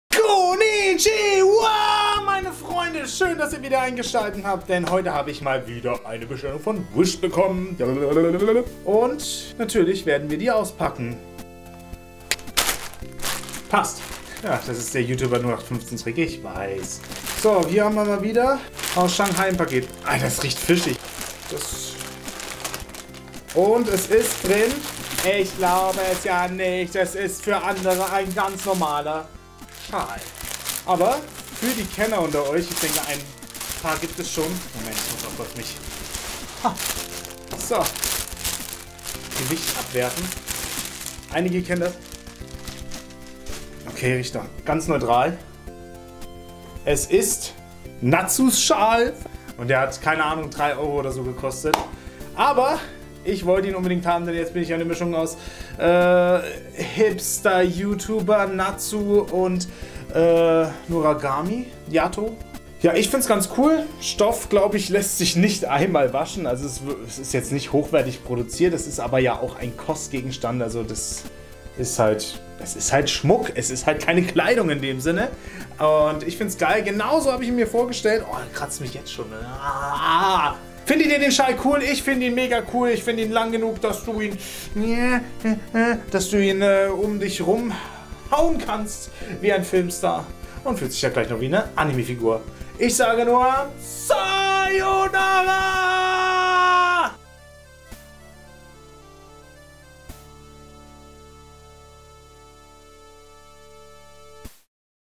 Wish Unboxing - Fairy Tail Schal
Es ist mal wieder soweit, ich habe ein Paket aus Asien bekommen! Ich habe mal wieder bei Wish zugeschlagen und mir etwas gekauft das ich schon lange haben wollte. Natsus Schal. War auch nicht wirklich teuer :D

Meine Kamera:

Meine Gopro:

Meine Belichtung und GreenScreen:

Meine Webcam für OBS ect. :

Meine Mikro für OBS und Podcats:

Meine Externe Soundkarte fürs Mic:

Meine Konsolen Capture Karte von Elgato: